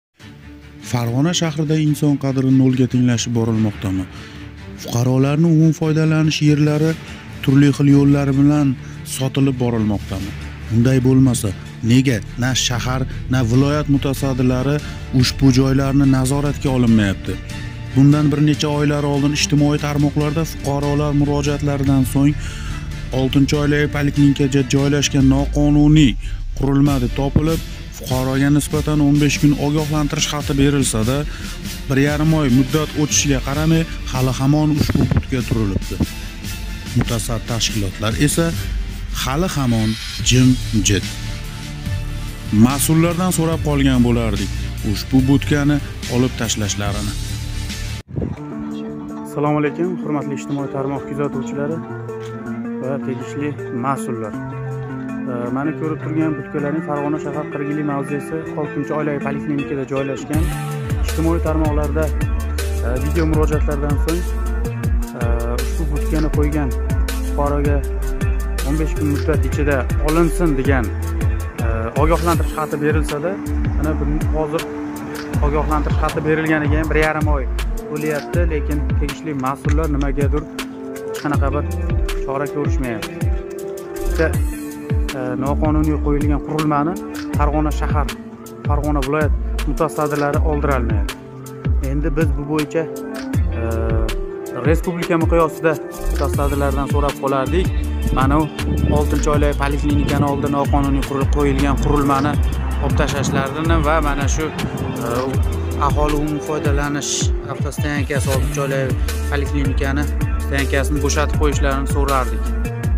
Farg'ona Viloyati Farg'ona shaharda qachon qonunlar ishlaydi?
FARG'ONA SHAHARNING AYRIM HUDUDLARIDA INSON HUQUQLARI, INSON MANFAATLARI BORA, BORA NO'LGA TENGLASHIB KELMOQDA.
XUSUSAN FARG'ONA SHAHAR KIRGULI MAVZESIDA JOYLASHGAN 6-OYLAVIY POLIKLINIKA OLDI QISMI FUQAROLAR UCHUN AVTO STAYANKASINI NOQONUNIY YO'LLAR BILAN EGALANIB OLISHSA HAM BARCHA MUTASADI RAXBARLAR BEPARVOLIK QILISHAYOTGANGA O'XSHAYDI. AXOLI UMUMIY FOYDALANISH YERLARI HAM BUGUNGI KUNDA PULLANIB, SAVDOLANIB TURLI YO'LLAR BILAN EGALANMOQDAMIKIN? AGAR UNDAY  BO'LMASA NEGA USHBU VIDEODAGI BUTKANI HECHQAYSI BIR DAVLAT ORGANLARI NAZORATGA OLIB, USHBU BUTKANI BU YERDAN OLISH CHORASI KO'RILAYOTGANI YO'Q? YUQORIDA TAKITLAB O'TILGANIDEK INSON QADRI, FUQAROLAR  MANFATINI HECHKIM HIMOYA QILMAYAPDIMIKIN? SHU O'RINDA BIZ RESPUBLIKA MIQYOSIDA ISHCHI GURUX MASULLARIGA UMID QILIB MUROJAAT QOLDIRAMIZ. AXOLINING YERLARNI TURLI XIL REDRLIK DEYDIMI? KORRUPSIYA YO'LIMI? XULLAS BIR SO'Z BILAN AYTGANDA AXOLI UMUMIY FOYDALANISH YERLARINI O'Z BOSHIMCHALIK BILAN EGALLAB OLINAYOTGAN JOYLARNI NAZORATGA OLISHLARINGIZNI VA YERLARNI BO'SHATIB QO'YISHLARIDA AMALIY YORDAM BERISHINGIZLARNI KUTIB QOLAMIZ .